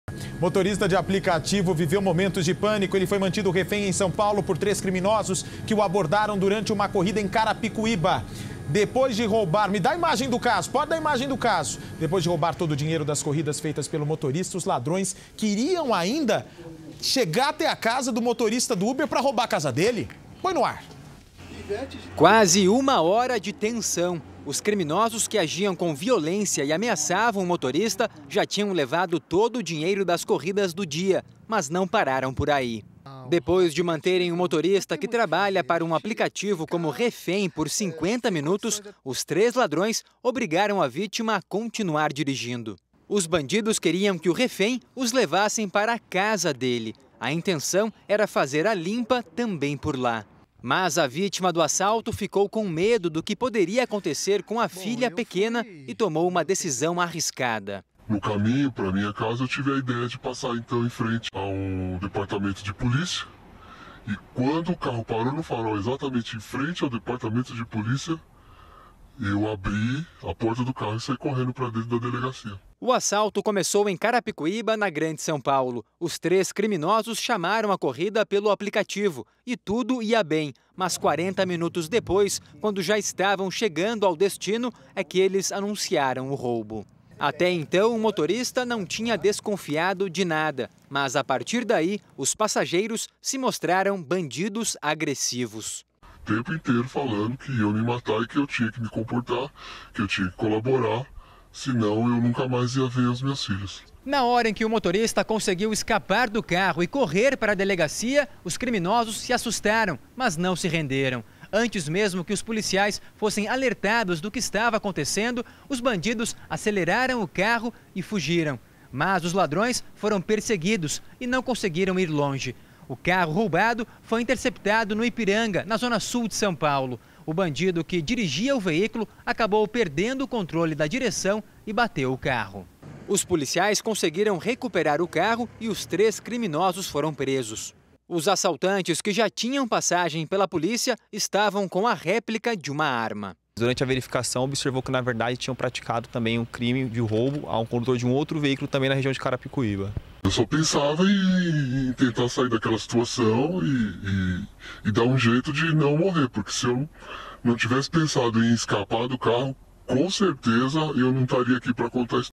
Motorista leva bandidos até a delegacia após ser assaltado em Carapicuíba (SP)
Os três bandidos, que pediram o carro pelo aplicativo em Carapicuíba, foram presos. A vítima foi mantida refém por cerca de 50 minutos.